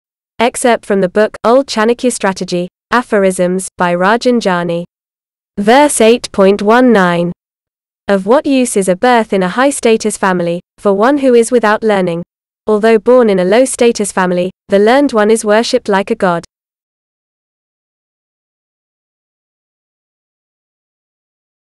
8.19 Old Chanakya Strategy: Aphorisms
Vriddha Chanakya Niti, translated as "Old Chanakya Strategy: Aphorisms" by Rajen Jani, is a book of aphorisms ascribed to Acharya Chanakya. It is a timeless Sanskrit classic and has universal appeal.

This book contains:
- 342 aphorisms
- Sanskrit original verse
- English transliteration
- English translation
- Commentary, wherever required
- Appendices for a wider understanding

A shining gem from the treasure trove of Sanskrit classical literature